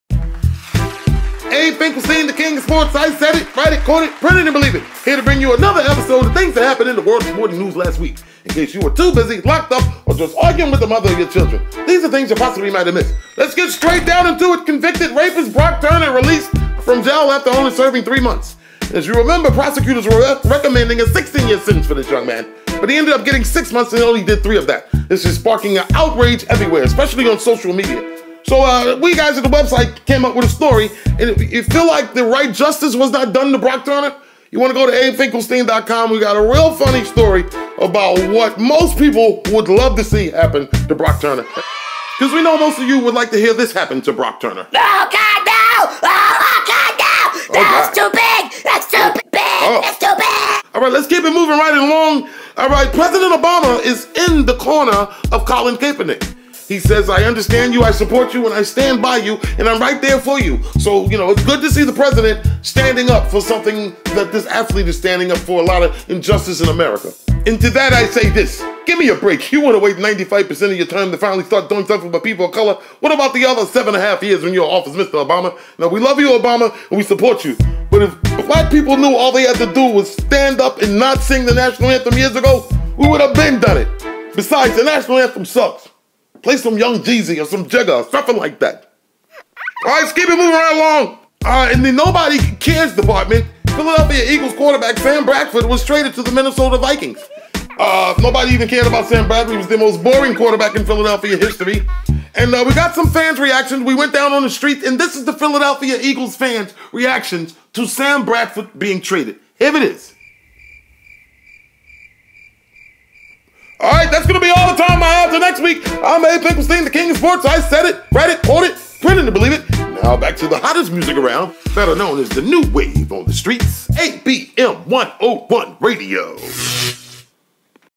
DID BROCK TURNER GET ATTACKED BY THE BOOTY WARRIOR?
Brock Turner gets what's coming to him, Pres Obama is Riding with Colin Kaepernick and Phila Eagles Fans React to The Sam Bradford to Minnesota Trade.